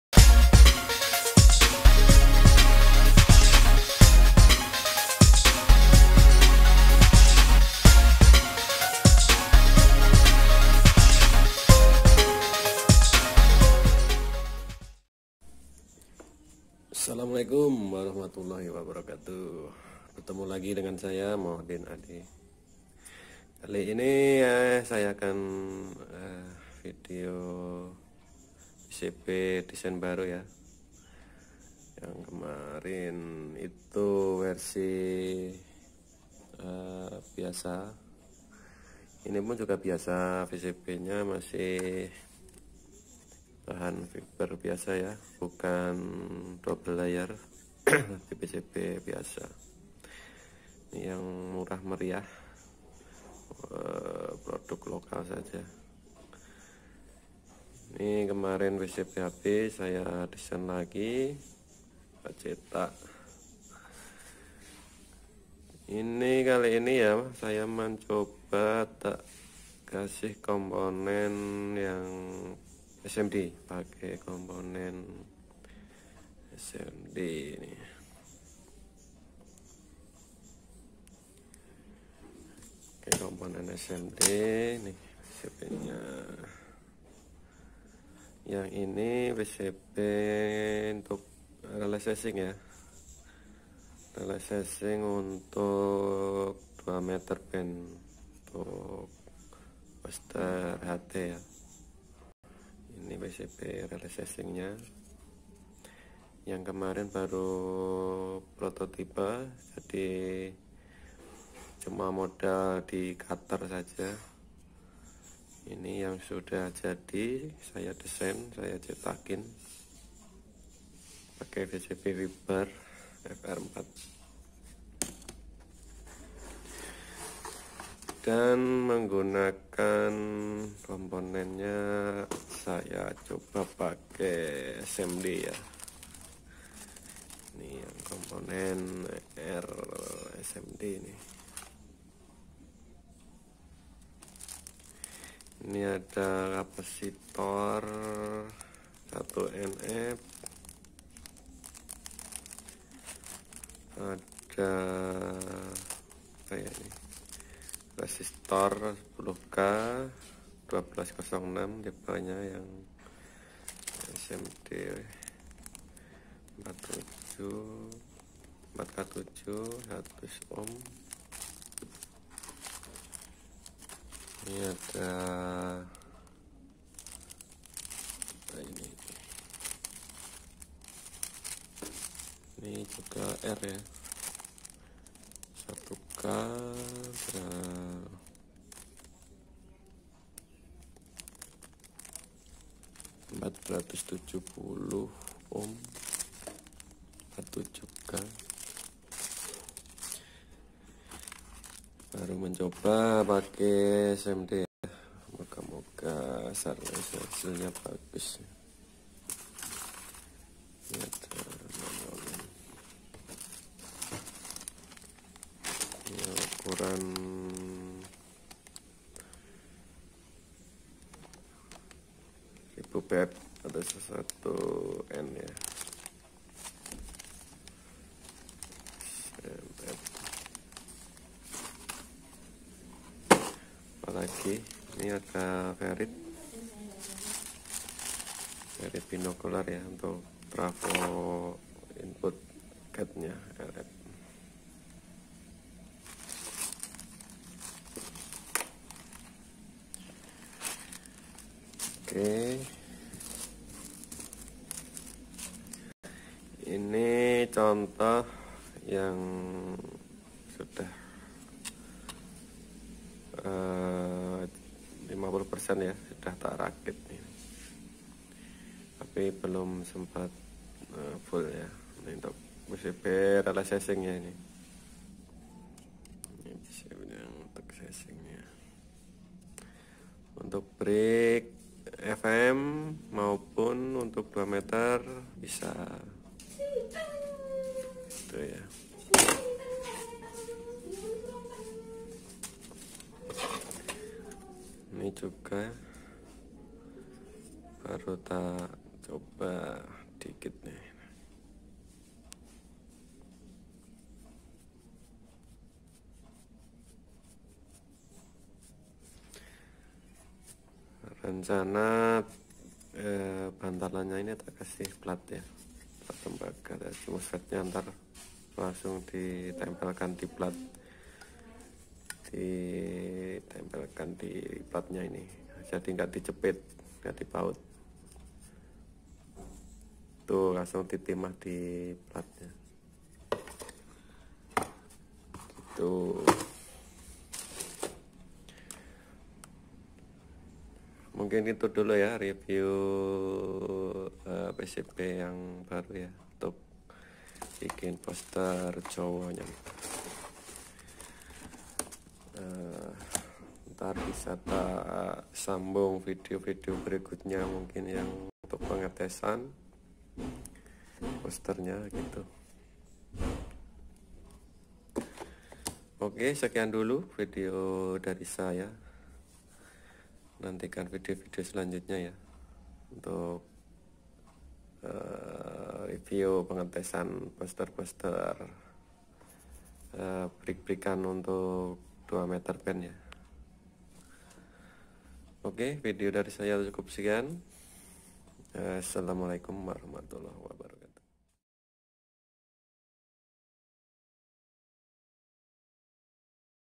PCB untuk keperluan boster vhf
Desain PCB lagi untuk boster vhf dan relay sesing nya..